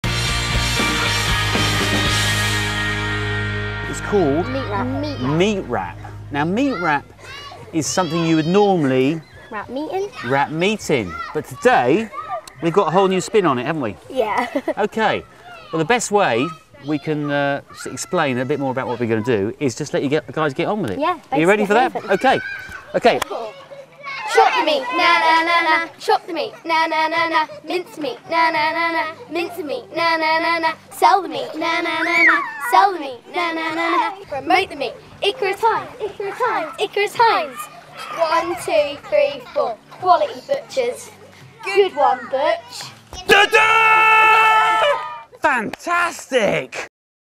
Meat rap
Icarus Hines of Cromer Norfolk with a little help does meat rap